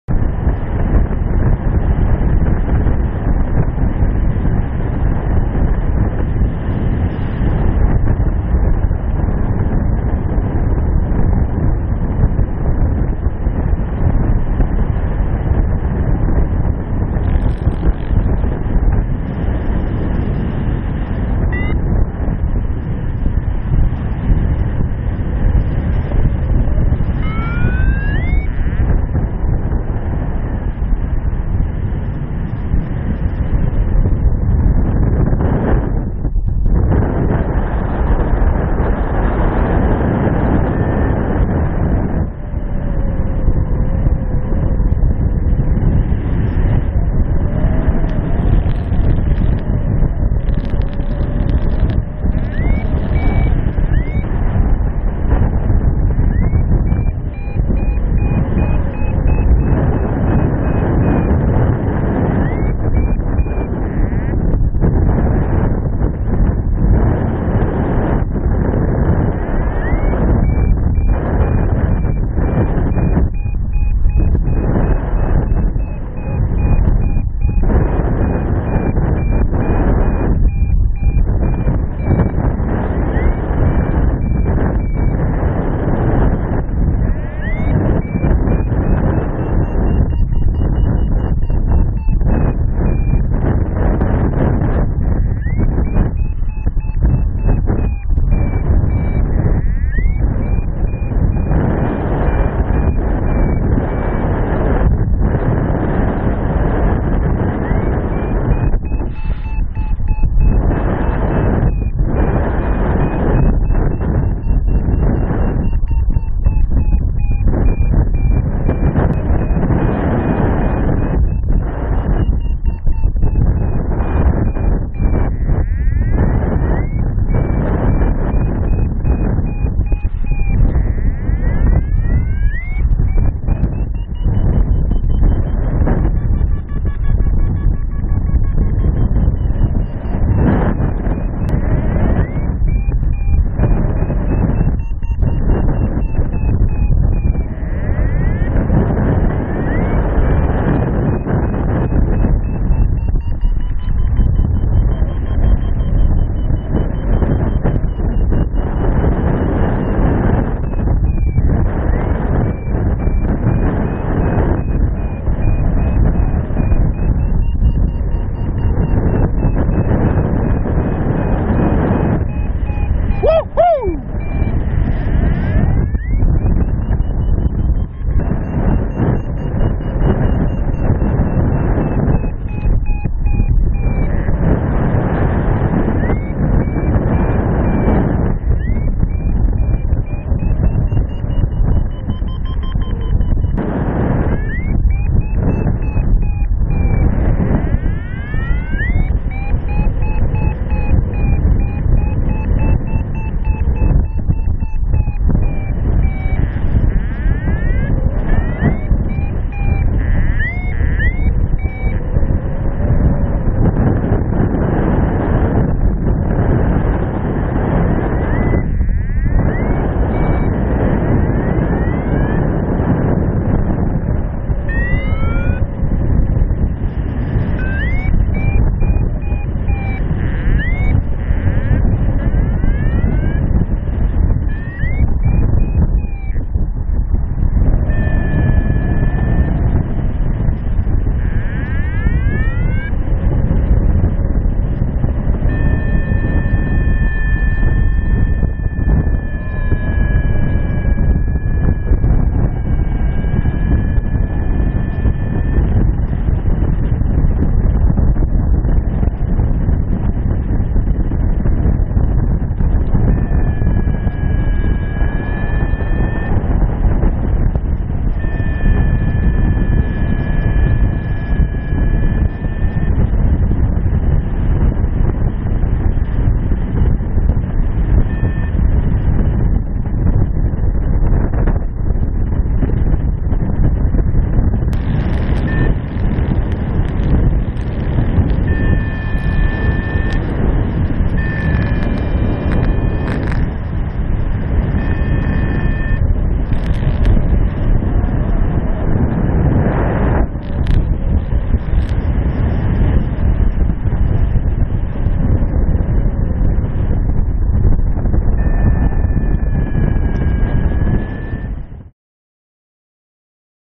Thermal through inversion
First thermal flight in 2016. Arrived a little late on take off. While hiking up the mountain I could see 2 gliders cruising at altitude so I knew that there must be a way to climb. The take off is at around 640m ASL. After taking off I met active air. I found several thermals that pushed me up to around 800m. At this level the air became turbulent and the lift disappeared. After about 10-15 minutes of "fishing" around I met a more vigorous thermal. At 800m I felt more turbulence but eventually I got through. Higher the thermal became smoother. I could climb to the airspace limit, 3500ft, 1067m. Once there I enjoyed the view and set as target a small XC flight. I am so happy that the season 2016 is on. Hope the next flying day will come soon!